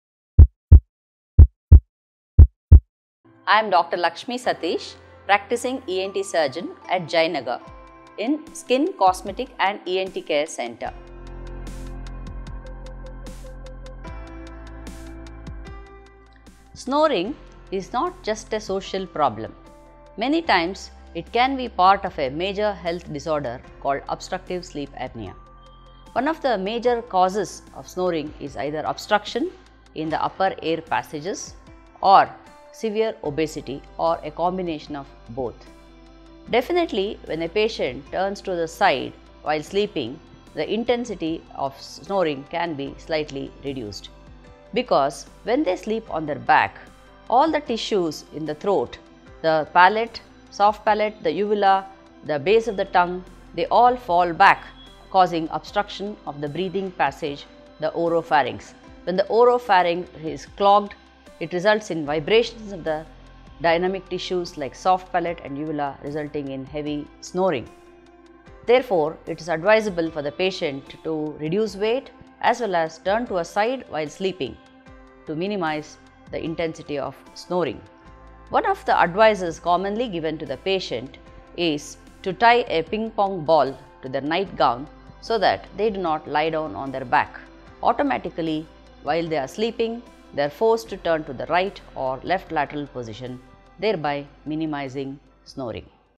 Will Sleeping on your side reduce Snoring? - Dr. Lakshmi Ponnathpur
Snoring is not just a social problem many times it can be part of a major health disorders called obstructive sleep apnea. One of the major causes of snoring is either obstruction in the upper air passages or severe obesity or a combination of both. Definitely when a patient turns to the side while sleeping the intensity of snoring can be slightly reduced because when they sleep on the back on the tissues in the throat the soft palate, the uvilla, base of the tongue they all fall back causing obstruction of the breathing passage, the oropharynx. When the oropharynx is blocked it results in vibration in the dynamic tissues like soft palate and uvilla  resulting in heavy snoring. Therefore it is advisable for the patient to reduce weight as well as turn to a side while sleeping to minimise the intensity of snoring. Advisors commonly given to the patient is to tie a Ping Pong ball to the night gown so that they do not lie down on their back, automatically while they are sleeping the first turn to the right or left lateral position there by minimizing snoring.